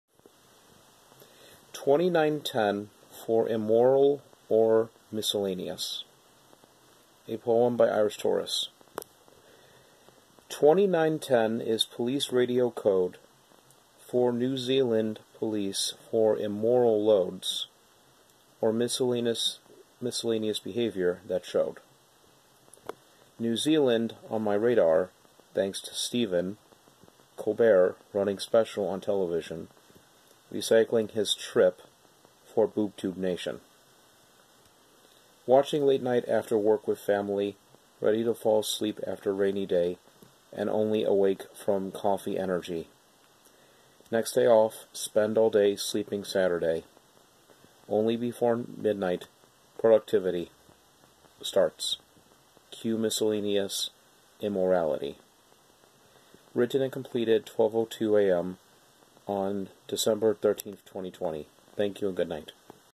29 10 for immoral or miscellaneous
Twenty-nine ten is police radio code
For New Zealand police for immoral loads
Or miscellaneous behavior that showed.

New Zealand on my radar thanks to Stephen
Colbert running special on television
Recycling his trip for boob tube nation.

Watching late night after work with family,
Ready to fall asleep after rainy day,
And only awake from coffee energy.

Next day off, spend all day sleeping Saturday.
Only before midnight, productivity
Starts. Cue miscellaneous immorality.
-12:02am, 12/13/20